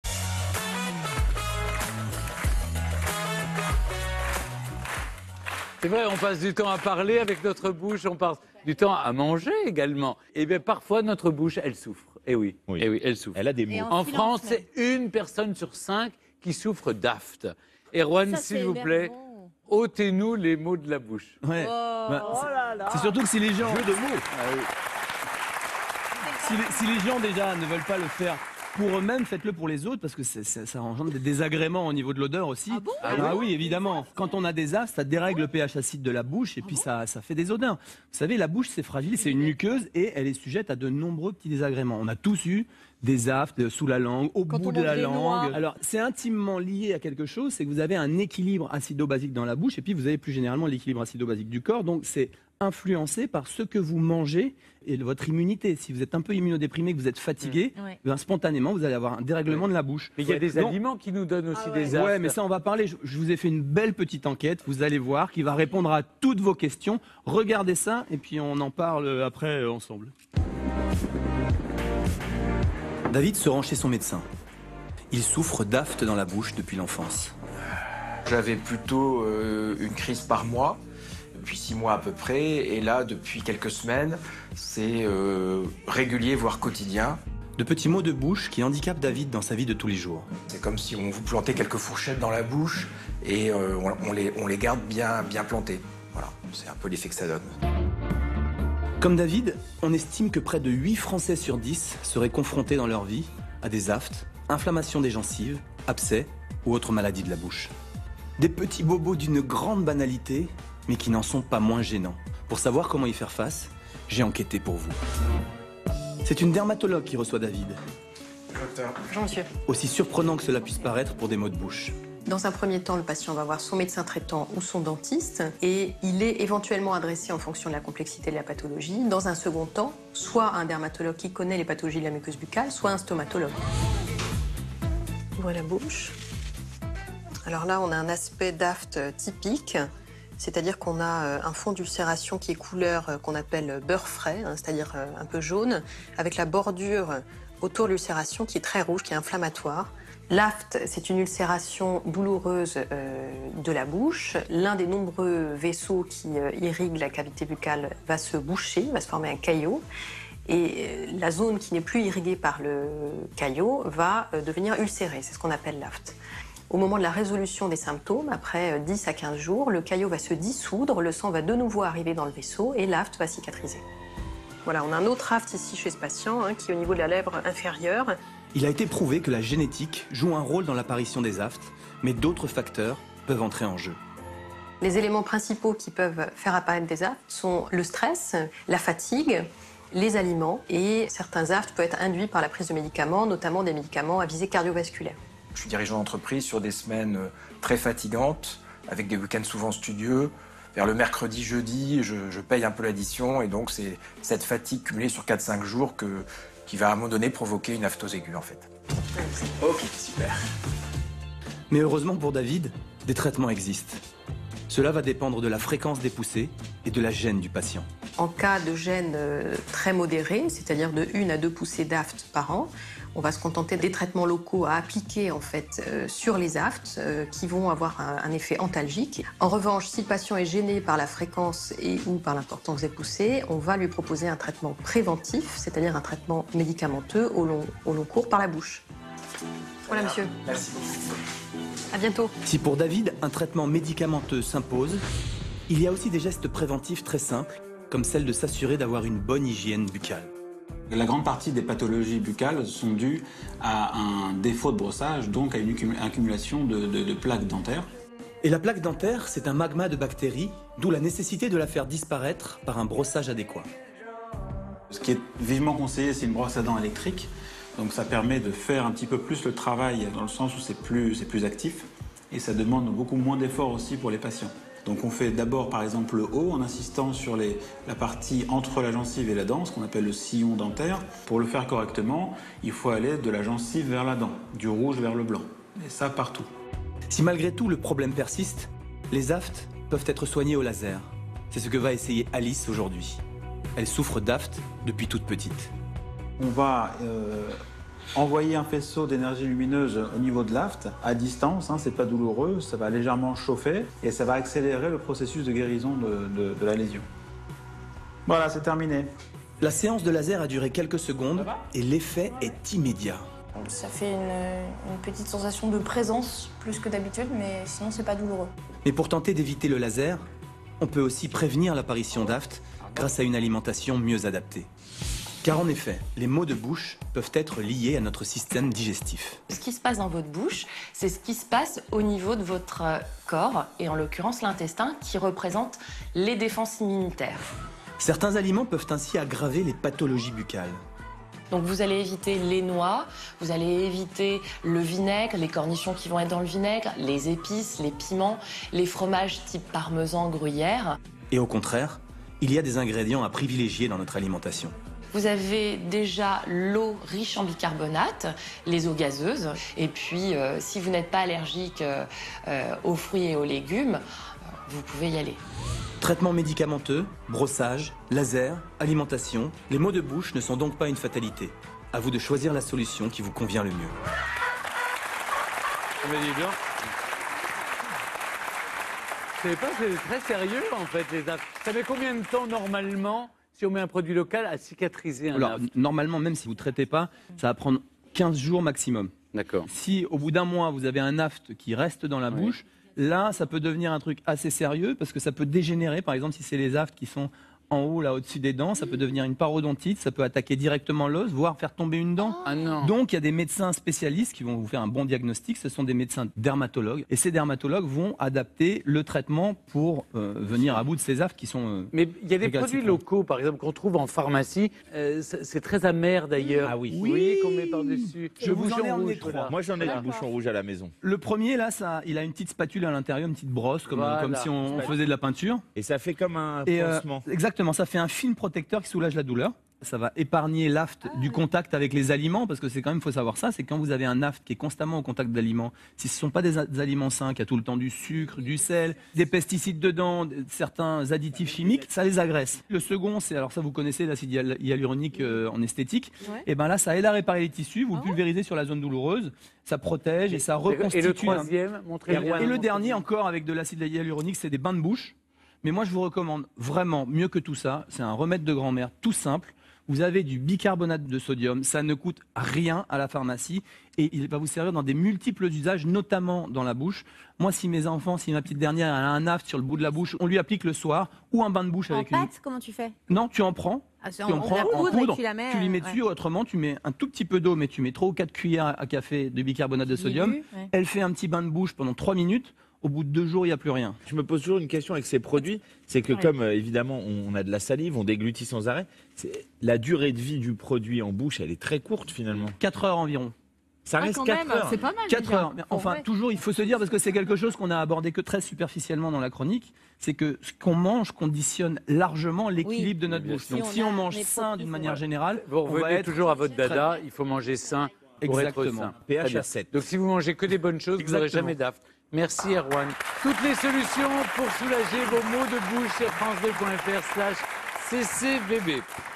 [BIEN-ÊTRE] Soigner les maux de bouche
Aux heures de repas, notre bouche a tout pour être heureuse, mais parfois, impossible pour elle de nous suivre. Et oui la pauvre souffre. Dévorer des yeux nos assiettes, n’est pas la solution et laisser les choses dégénérer encore moins. En France, c’est prêt d’une personne sur 5 qui souffre régulièrement d’aphtes. Ces maux de bouche d'une grande banalité n'en sont pas moins gênants car douloureux. Pour tout savoir sur ces petits maux de la bouche et surtout comment les traiter, regardez !